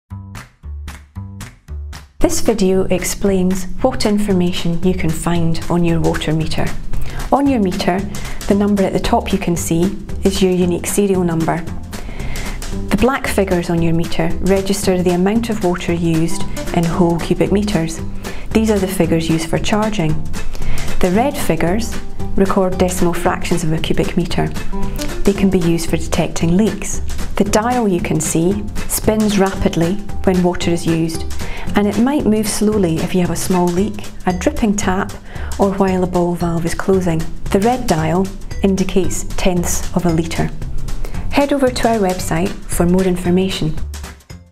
Water Meter Explained
A short video explaining how to read your water meter.